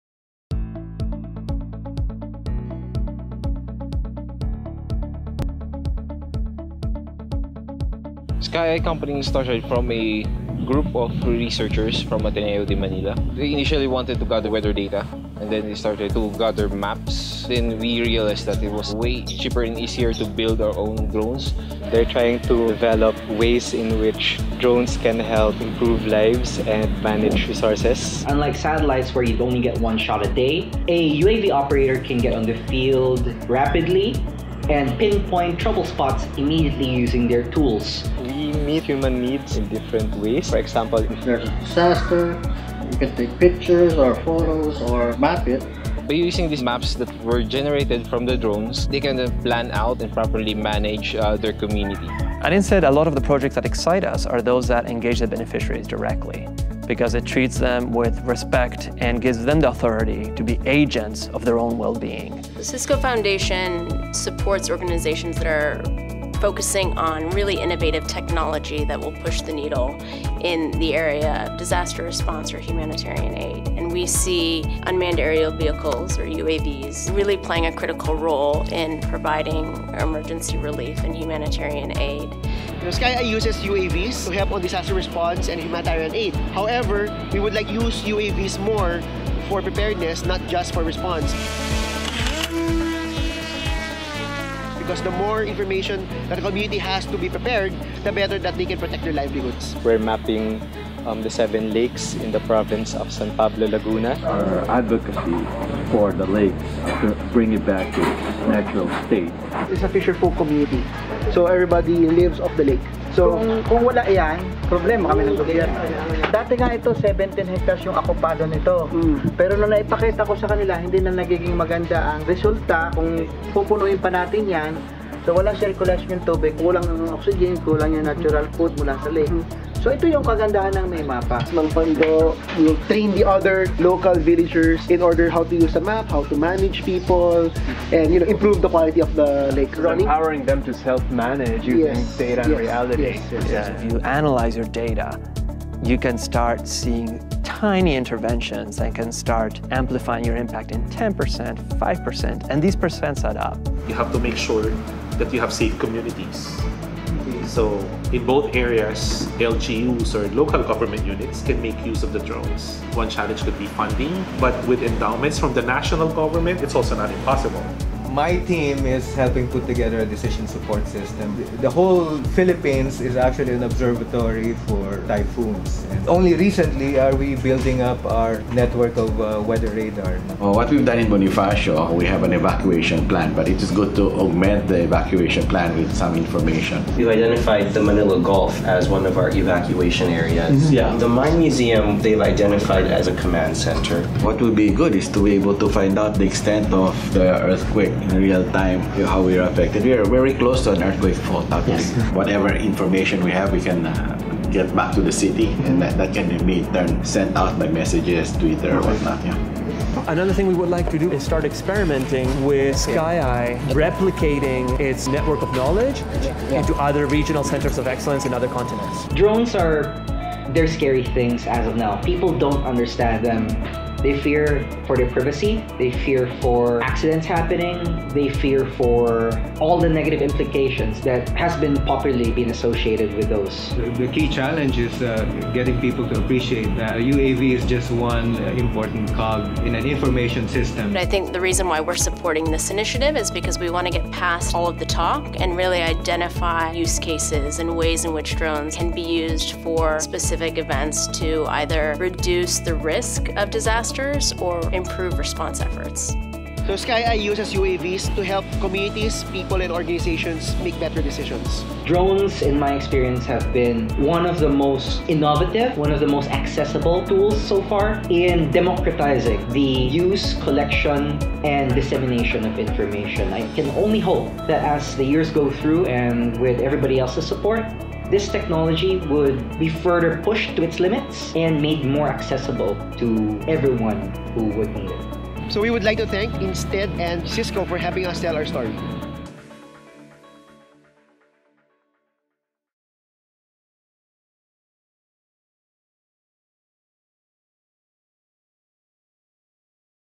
SkyEye: Drones For Humanitarian Response in Philippines
This year I had the opportunity to work with SkyEye Inc., InSTEDD and Cisco to create multiple videos about the application of UAVs in humanitarian response.

A variation of this edit was featured during an April 14th, 2016 event at Singularity University in Silicon Valley